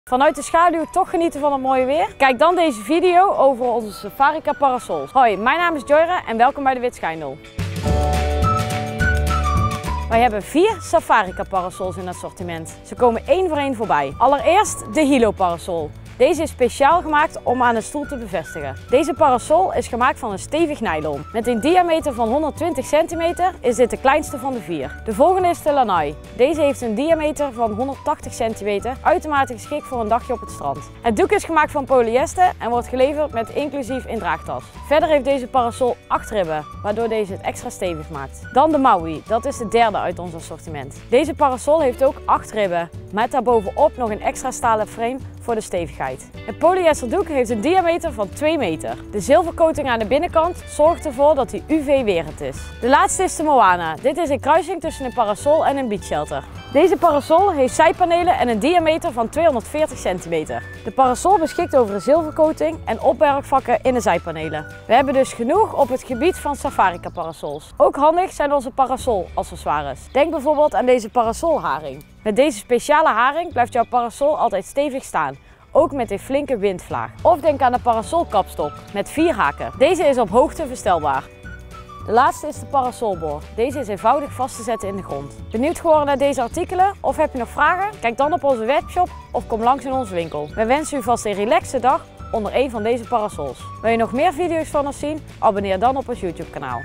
Safarica parasols - De Wit Schijndel
Vanuit de schaduw toch genieten van het mooie weer? Bekijk dan deze video over de Safarica parasols!

0:15 Safarica Hilo
0:25 Safarica Lanai: en
0:39 Safarica Maui: en
0:53 Safarica Moana
1:08 Safarica parasol accessoires